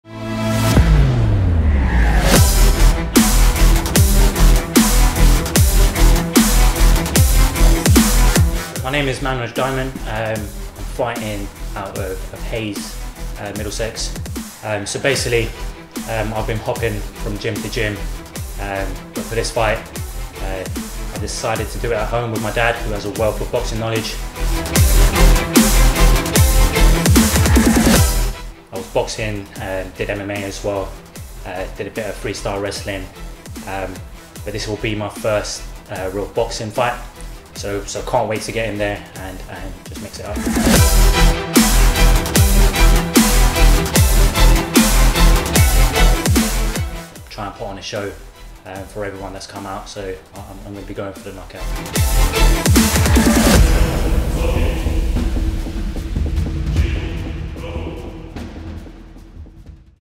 Future of Boxing - Manraj Dhiman - Fighter Profile
Future of Boxing and Light Circle Media promo featuring fighter, Manraj Dhiman training for his debut fight.